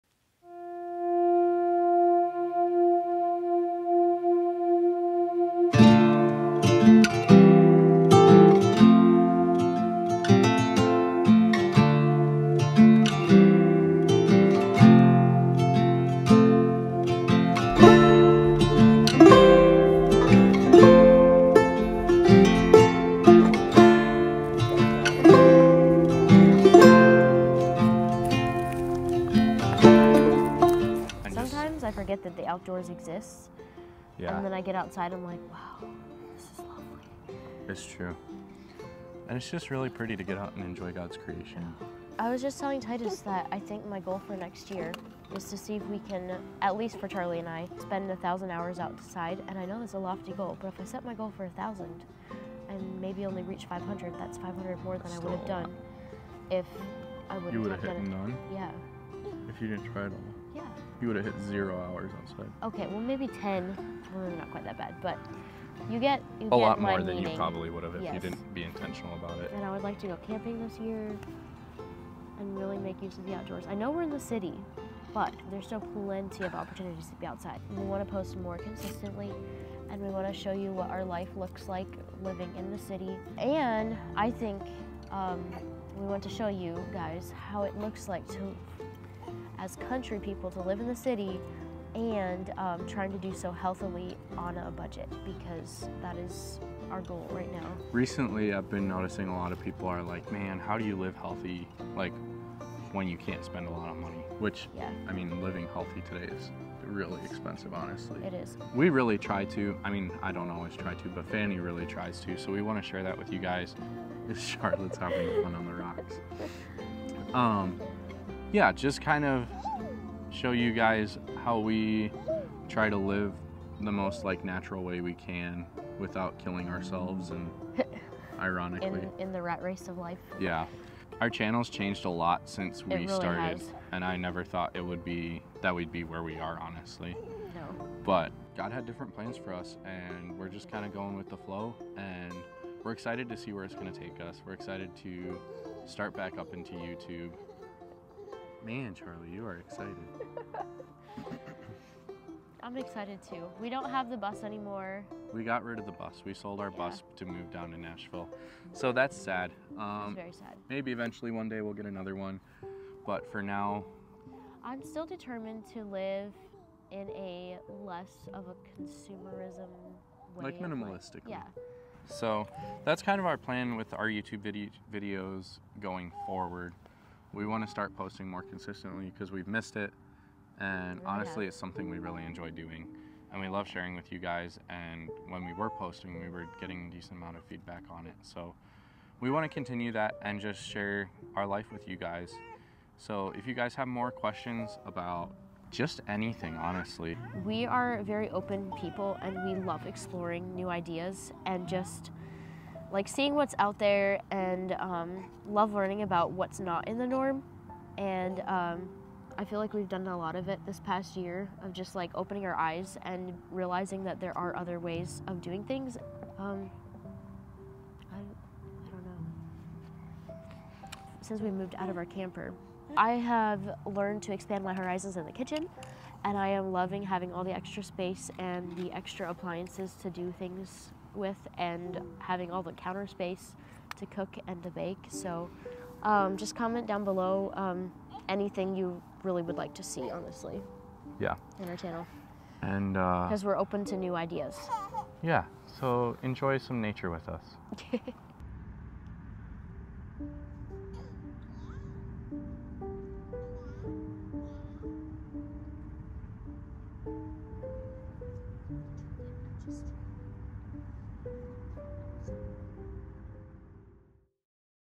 Where have we been and what's ahead!
Hey y'all! It has been so long since we posted a video, honestly almost a year. But we are back and so excited to start back up. We just wanted to share what we have been up to briefly and what our channel looks like moving forward! Hope y'all had a wonder 2023 and we are excited to share our 2024 season and all that will bring with you!